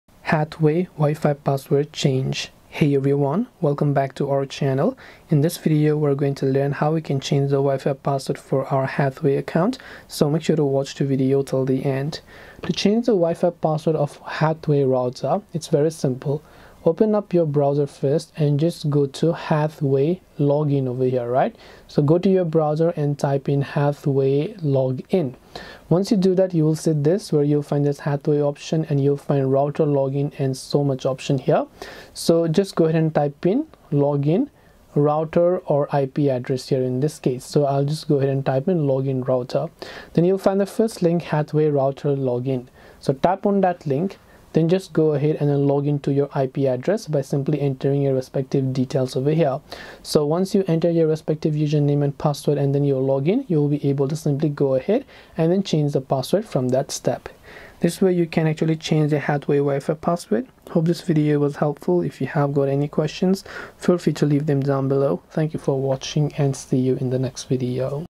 Hathway WiFi Password Change !
In this video, I will show you how to change your Hathway wifi password.

~ Chapters:
0:00 Introduction
0:12 Hathway WiFi Password Change
1:00 Conclusion